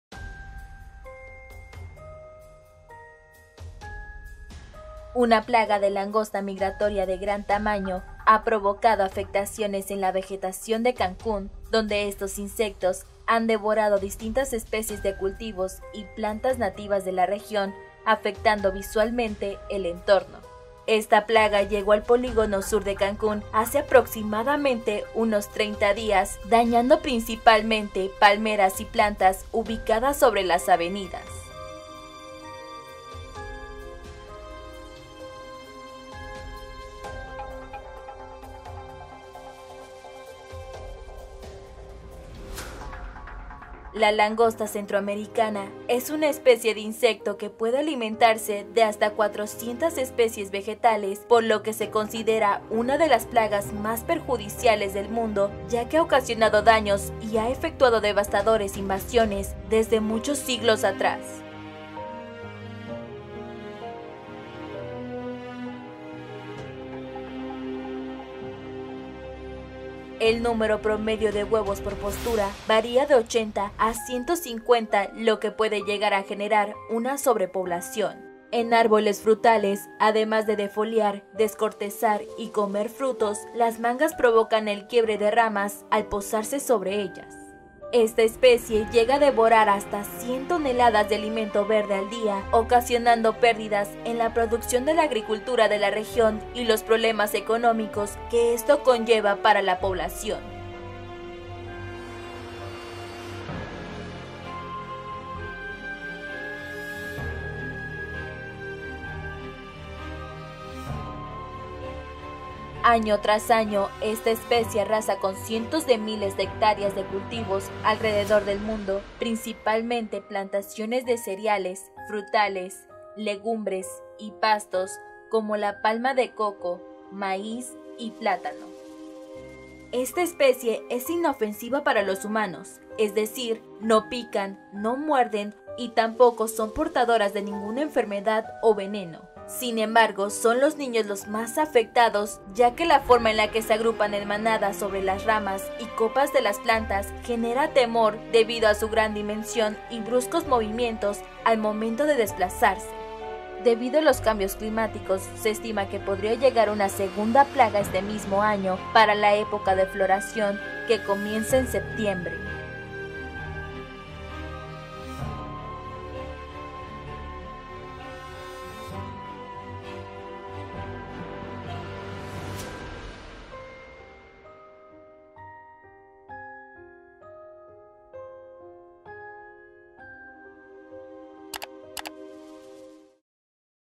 Plaga bíblica de langostas ataca vegetación en Cancún
Una plaga de saltamontes migratorias de gran tamaño ha provocado afectaciones en la vegetación devorado especies de cultivos y plantas nativas de la región 😱😰🍃🦗